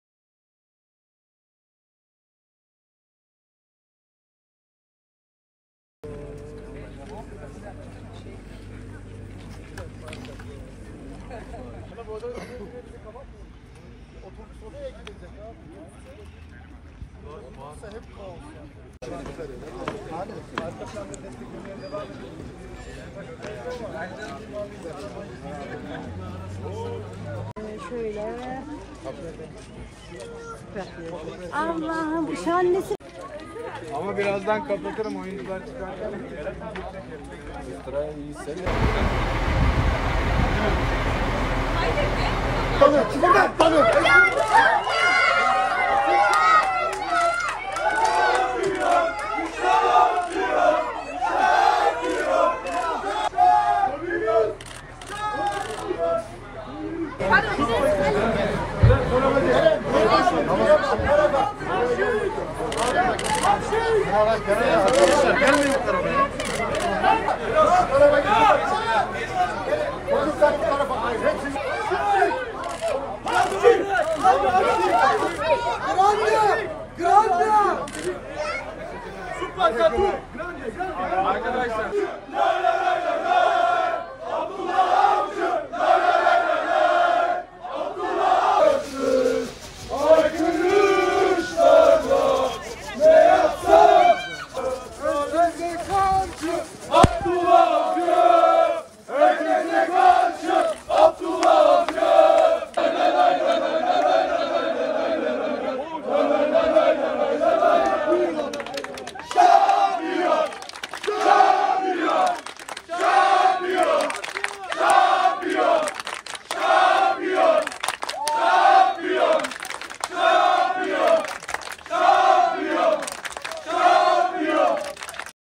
Trabzonspor'u Avusturya'da taraftarı yalnız bırakmadı
- Avusturya kampındaki ilk hazırlık maçında İtalya'nın Torino takımı ile karşılaşacak bordo-mavililer, müsabakanın oynanacağı kasabaya taraftar konvoyu eşliğinde geldi

TIROL (AA) - Yeni sezon hazırlıkları kapsamında Avusturya'da kamp yapan Trabzonspor'u, İtalya'nın Torino takımı ile oynayacağı maç öncesi taraftarı yalnız bırakmadı.

Bordo-mavili ekibin Bad Haring Bölgesi'nde kamp yaptığı otele gelen yaklaşık 200 kişilik taraftar grubu, takıma otelden çıkışında sevgi gösterilerinde bulundu.

Ellerindeki meşaleleri yakarak teknik direktör Abdullah Avcı ve futbolculara tezahüratlar yapan taraftarlar, yaklaşık 30 kilometre uzaklıktaki Kitzbühel kasabasına takımlarını konvoy eşliğinde getirdi.

Trabzonspor'un Kitzbühel Stadı'nda oynanacak karşılaşmasına da çok sayıda taraftar geldi. 5 binin üzerinde bordo-mavili taraftar, karşılaşmayı izleyecek.